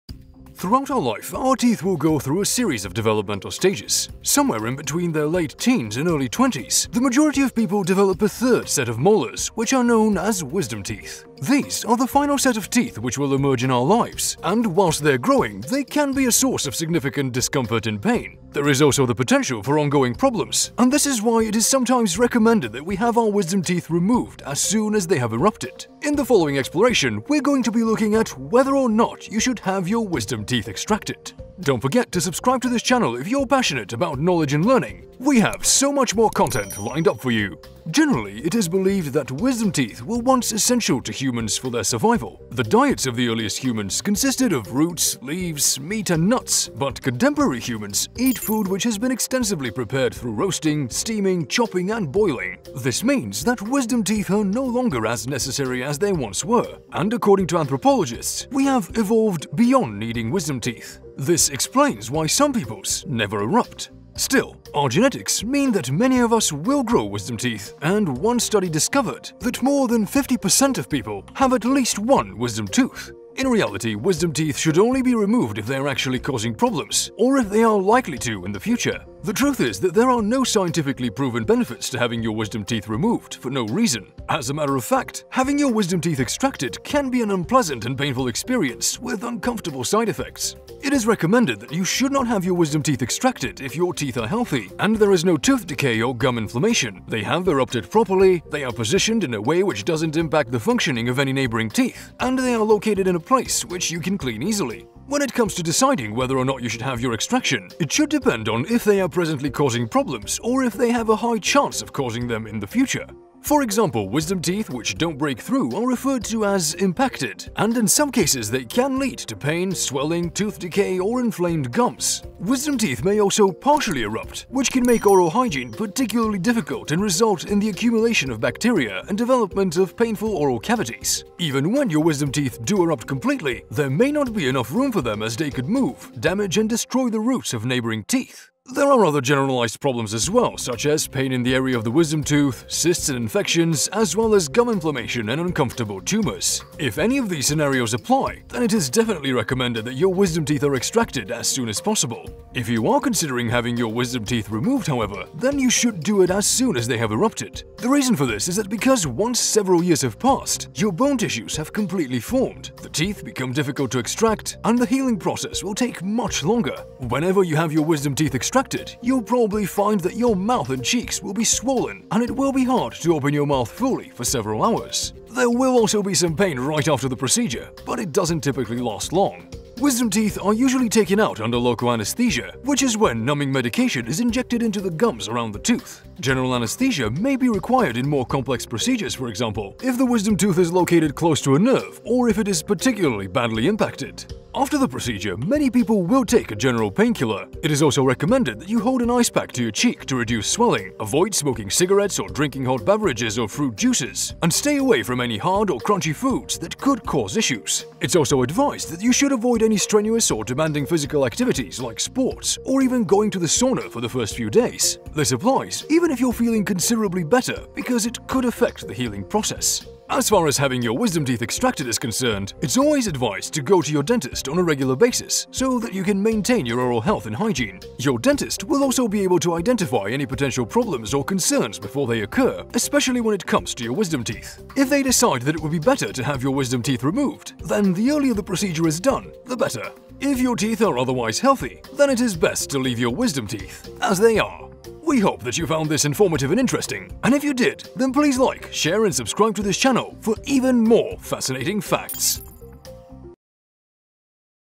Why You Should Not Extract Your Wisdom Teeth
Throughout our life, our teeth will go through a series of developmental stages. Somewhere in between their late teens and early twenties, the majority of adults will develop a third set of molars which are known as wisdom teeth. These are the final set of teeth which will emerge in our lives, and whilst they are growing they can be a source of significant discomfort and pain. There is also the potential for ongoing problems, and this is why it is sometimes recommended that we have our wisdom teeth removed as soon as they have erupted. In the following exploration, we’re going to be looking at whether or not you should have your wisdom teeth extracted.